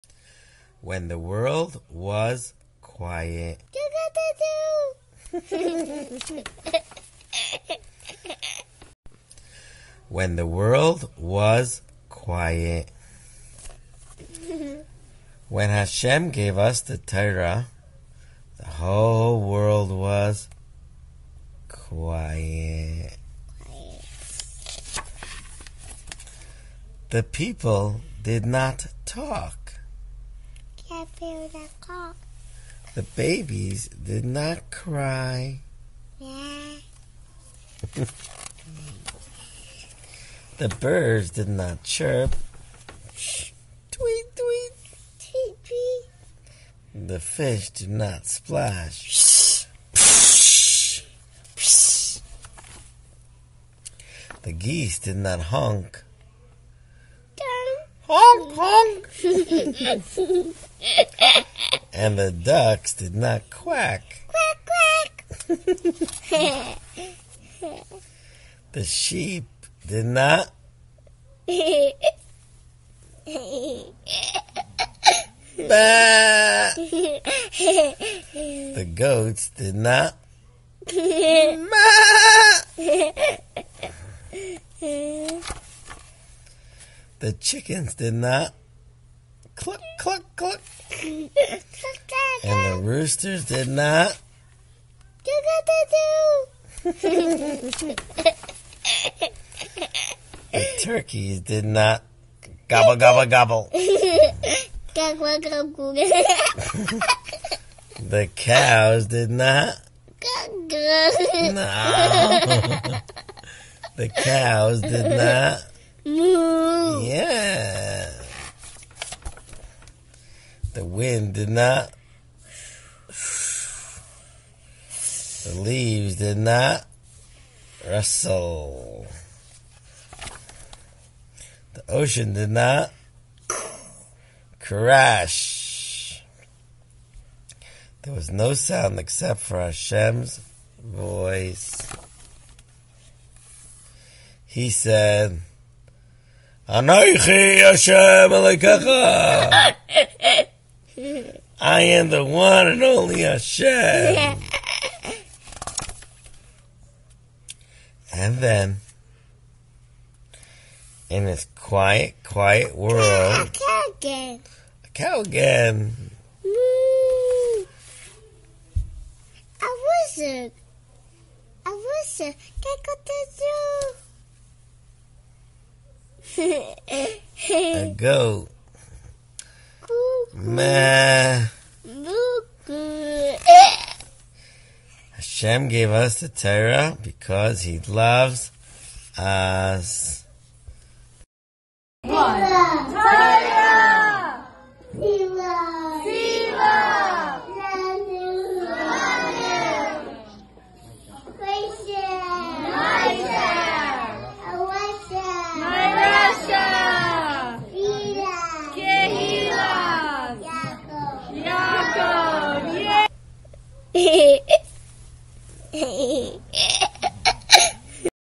When the world was quiet - Storytime with Sholom A"H
This video is about a book called "When The Torah was Given", written by Phyllis Nutkis and illustrated by Patti Arguillo. It was published and copyrighted in 2004 by Hachai Publishing. We have been granted permission to use it.

On February 23, 2022, the book was read to Sholom by @MenachemTauber. Sholom was captivated by the animal sounds and enjoyed participating in the story.

We kindly request you to consider contributing towards a Torah we are commissioning in memory of Sholom Tauber.